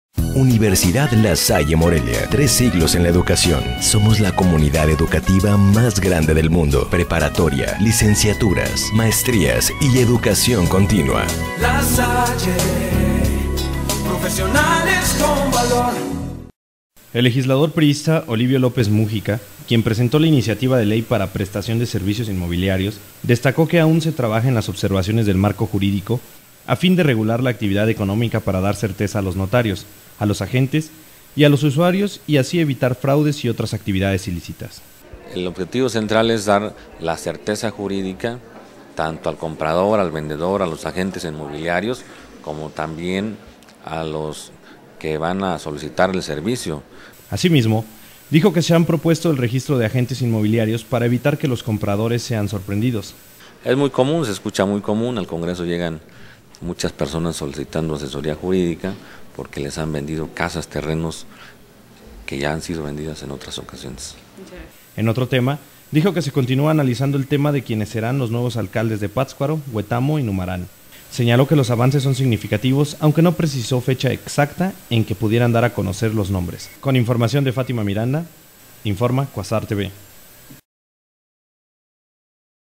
En análisis iniciativa de ley para prestación de servicios inmobiliarios
El legislador priísta, Olivio López Múgica, quien presentó la iniciativa de Ley para Prestación de Servicios Inmobiliarios, destacó que aún se trabaja en las observaciones del marco jurídico a fin de regular la actividad económica para dar certeza a los notarios, a los agentes y a los usuarios y así evitar fraudes y tras actividades ilícitas…